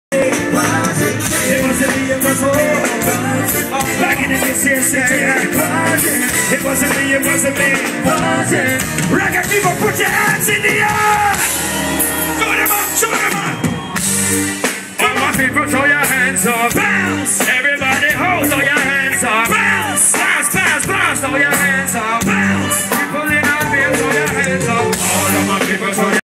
Shaggy at Festival Africa - Caribe concert in Almere 2012
Shaggy singing live at Festival Africa-Caribe concert in Almere (The Netherlands)